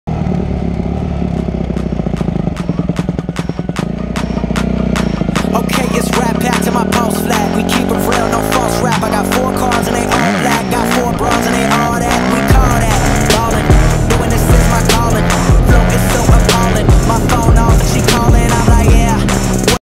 YZ 250F Wheelie
yz250f dirt bike wheelie down street  and I could have went longer but I have no 5th gear because my clutch is SLIPIN